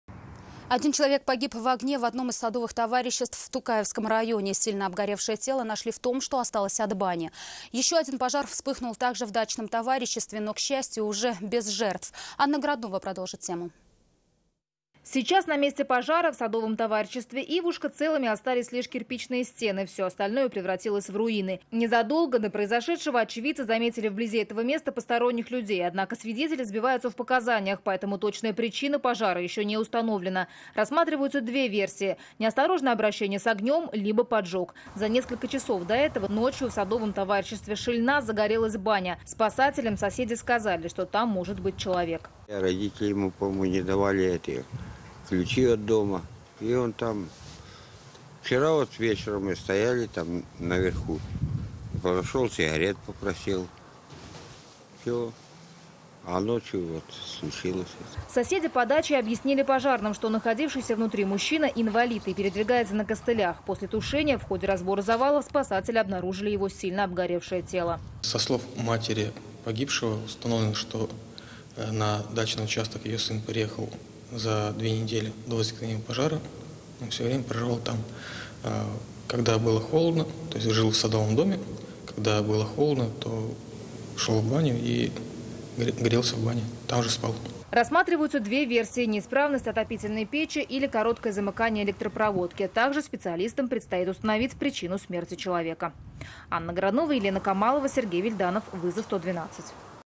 Один человек погиб в огне в одном из садовых товариществ в Тукаевском районе - ТНВ
Сильно обгоревшее тело нашли в том, что осталось от бани. Ещё один пожар вспыхнул так же в дачном товариществе. Но к счастью, уже без жертв.

Таяну ноктасы. В эфире: пн, вт, ср, чт - 19:00

7 дней. В эфире: вс - 19:30, 21:30

Координаты:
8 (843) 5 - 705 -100